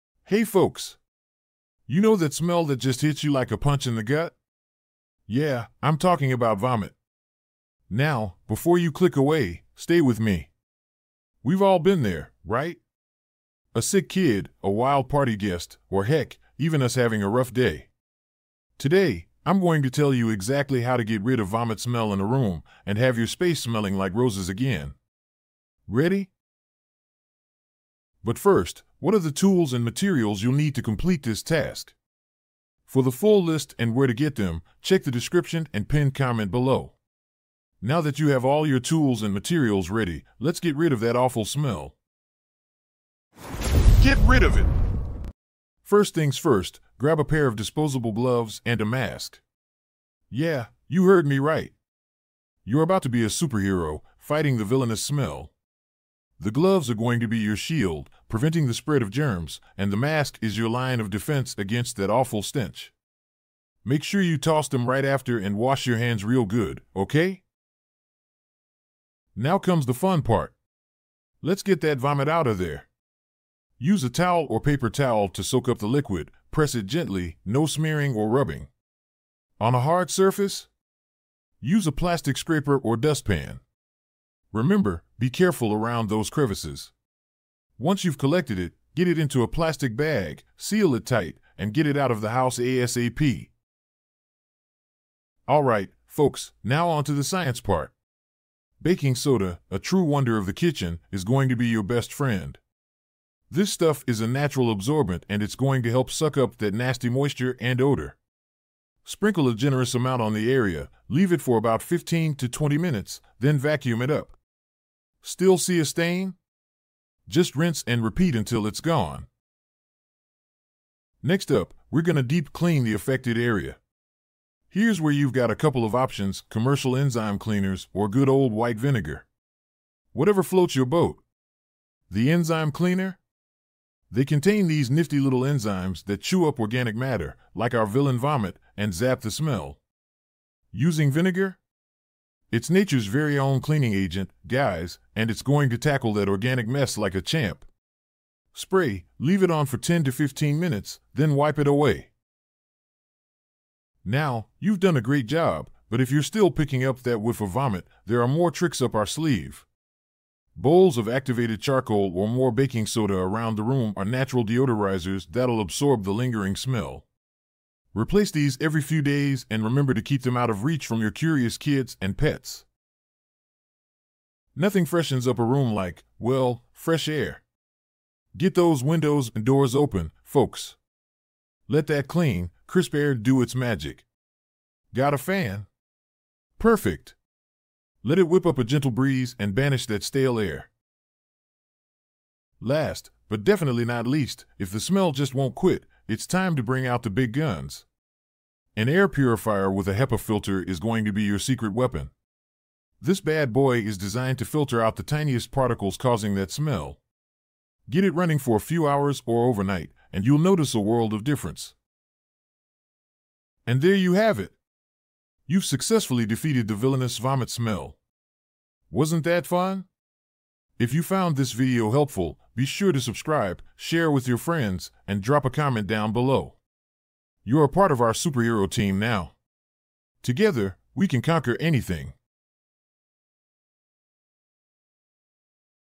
How to Get Rid of Vomit Smell in a Room - Fast & Easy!
I'm going to tell you exactly how to get rid of vomit smell in a room, and have your space smelling like roses again. Ready?

Paper towel
Plastic bag
Vacuum cleaner
Baking Soda
White vinegar
Enzyme cleaner
Activated charcoal
Air purifier with HEPA filter

Now that you have all your tools and materials ready, let's get rid of that awful smell...

First things first, grab a pair of disposable gloves and a mask. Yeah, you heard me right. You're about to be a superhero, fighting the villainous smell. The gloves are going to be your shield, preventing the spread of germs, and the mask is your line of defense against that awful stench. Make sure you toss them right after and wash your hands real good, okay?

Now comes the fun part. Let's get that vomit outta there! Use a towel or paper towel to soak up the liquid, press it gently, no smearing or rubbing. On a hard surface? Use a plastic scraper or dustpan. Remember, be careful around those crevices. Once you've collected it, get it into a plastic bag, seal it tight, and get it out of the house ASAP.

Alright, folks, now onto the science part. Baking soda, a true wonder of the kitchen, is going to be your best friend. This stuff is a natural absorbent and it's going to help suck up that nasty moisture and odor. Sprinkle a generous amount on the area, leave it for about 15 to 20 minutes, then vacuum it up. Still see a stain? Just rinse and repeat until it's gone.

Next up, we're gonna deep clean the affected area. Here's where you've got a couple of options - commercial enzyme cleaners or good old white vinegar. Whatever floats your boat. The enzyme cleaner? They contain these nifty little enzymes that chew up organic matter, like our villain vomit, and zap the smell. Using vinegar? It's nature's very own cleaning agent, guys, and it's going to tackle that organic mess like a champ. Spray, leave it on for 10 to 15 minutes, then wipe it away.

Now, you've done a great job, but if you're still picking up that whiff of vomit, there are more tricks up our sleeve. Bowls of activated charcoal or more baking soda around the room are natural deodorizers that'll absorb the lingering smell. Replace these every few days and remember to keep them out of reach from your curious kids and pets.

Nothing freshens up a room like, well, fresh air! Get those windows and doors open, folks. Let that clean, crisp air do its magic. Got a fan? Perfect! Let it whip up a gentle breeze and banish that stale air.

Last, but definitely not least, if the smell just won't quit, it's time to bring out the big guns. An air purifier with a HEPA filter is going to be your secret weapon. This bad boy is designed to filter out the tiniest particles causing that smell. Get it running for a few hours or overnight, and you'll notice a world of difference.

And there you have it! You've successfully defeated the villainous vomit smell. Wasn't that fun? You're a part of our superhero team now. Together, we can conquer anything!